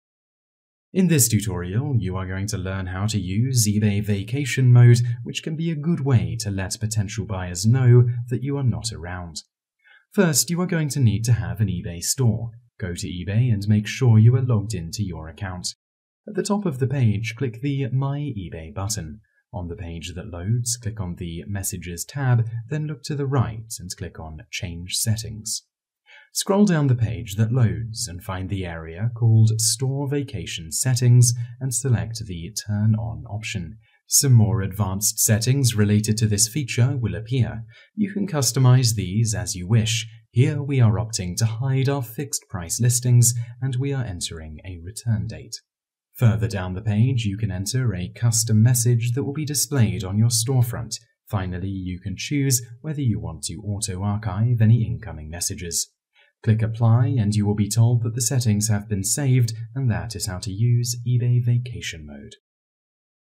How to eBay Vacation Setting
This tutorial will show you how to use vacation mode on eBay.

In this tutorial you are going to learn how to use eBay vacation mode which can be a good way to let potential buyers know that you are not around.

Step # 1 -- Navigating to Change Settings
First you are going to need to have an eBay store. Go to eBay and make sure that you are logged into your account. At the top of the page click the "My eBay" button. On the page that loads click on the "Messages" tab then look to the right and click on "Change settings".

Step # 2 -- Entering a Return Date
Scroll down the page that loads and find the area called "Store vacation settings" and select the "Turn on" option, some more advanced settings related to this feature will appear. You can customize these as you wish -- here we are opting to hide our fixed price listings and we are entering a return date.

Step # 3 -- Entering a Custom Message
Further down the page you can enter a custom message that will be displayed on your store front. Finally you can choose whether you want to auto archive any incoming messages. Click "Apply" and you will be told that the settings have been saved and that is how to use eBay vacation mode.